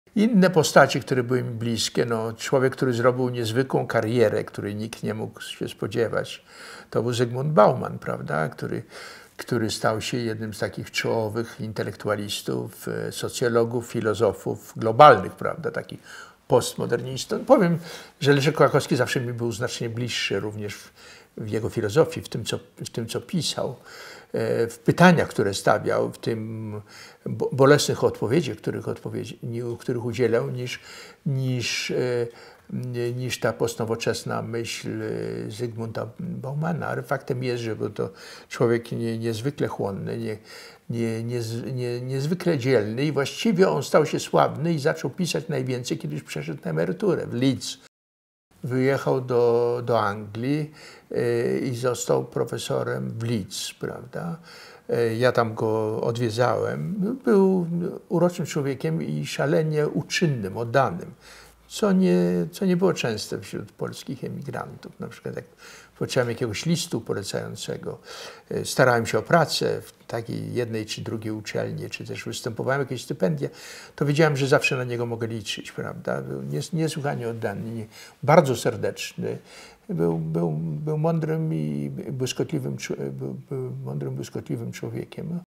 Aleksander Smolar - Zygmunt Bauman (179/201)
Aleksander Smolar (b. 1940) is a Polish writer, political activist and adviser, vice-president of the Institute for Human Sciences and president of the Stefan Batory Foundation. [Listener: Vitek Tracz]

TRANSCRIPT: There were other people to whom I was close. There was one man who had an extraordinary career which nobody could have predicted, and that was Zygmunt Bauman who became a leading intellectual, a sociologist, a philosopher on a global scale, a postmodernist. I have to say that I found Leszek Kołakowski, his philosophy and what he wrote, the questions he asked, the painful answers he gave far more in keeping with my thoughts than the postmodern ideas of Zygmunt Bauman. However, he was an exceptionally courageous and receptive person whose writings and fame increased once he retired to Leeds. He left quite soon for England and became a professor in Leeds. I would visit him there. He was a charming man, extremely helpful and dedicated which was uncommon among Polish émigrés. For example, whenever I needed a letter of recommendation for when I was applying for a job in one or another university or for a grant, I knew that I could always count on him. He was an extremely dedicated, sincere, wise and bright man.